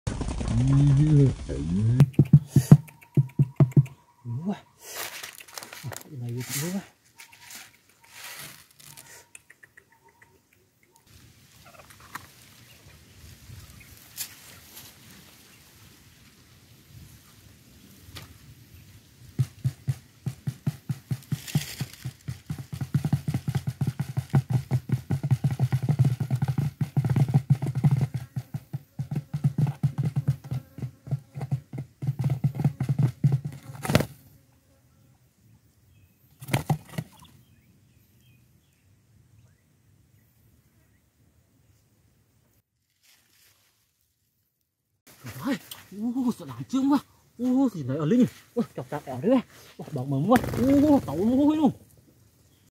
Awesome Create Underground Quail Trap
In this video, we will show you how to create an awesome underground quail trap using simple materials. We will guide you through each step of the process, from digging the trap to setting it up for maximum effectiveness. Watch and learn how to efficiently catch quail in your backyard or hunting grounds with this innovative and practical trap design.